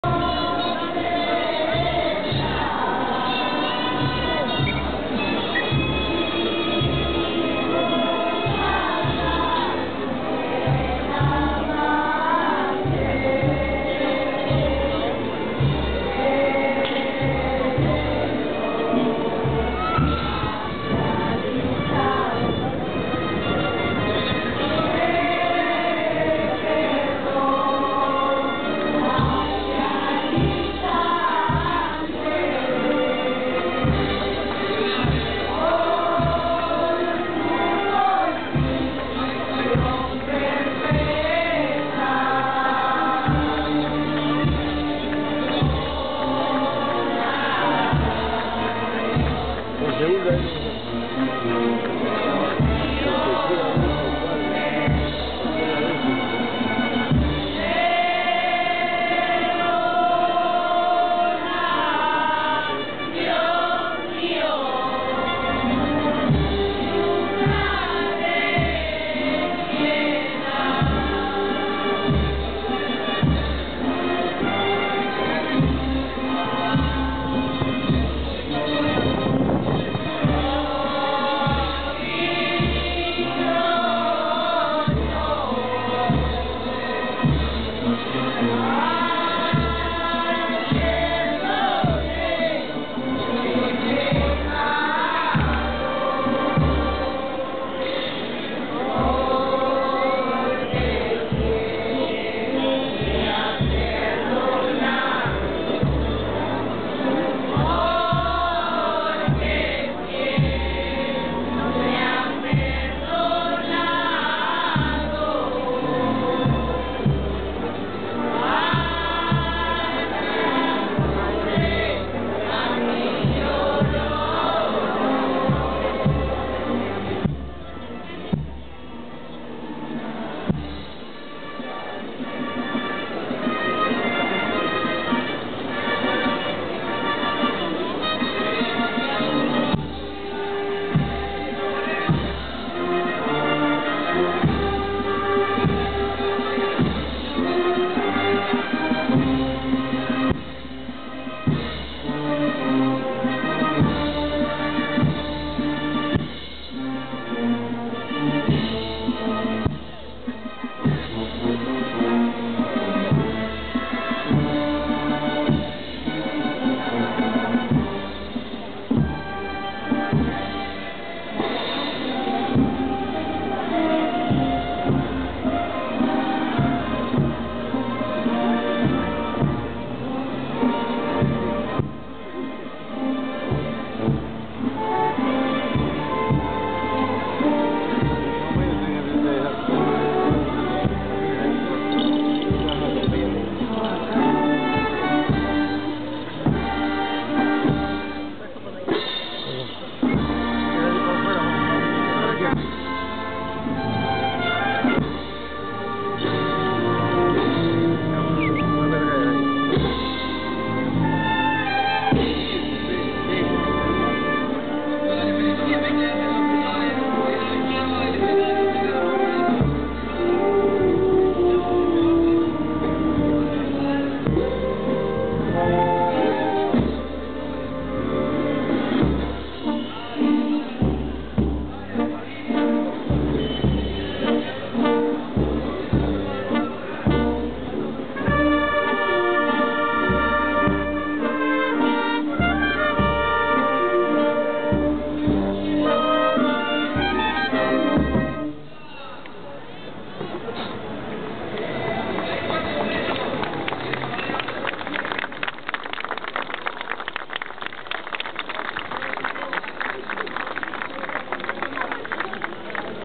Peru-Lima-Procesión del Señor de los Milagros
Procession of the Lord of Miracles near Hospital del Niño (the childrens hospital) in Lima, Peru, Avenida Brazil. Tuesday October 19, 2010. HzF People walk backwards in devotion

Lima, Peru
Processie van de Heer van de Wonderen nabij het Hospital del Niño (het kinderziekenhuis) in Lima, Peru, Avenida Brazil. Op dinsdag 19 oktober 2010. HzF. (alias Petrus Puck, vader van Pietje Puk)

La procesión del Señor de los Milagros a Lima, Peru es una tradicion annual. Muy impressionante.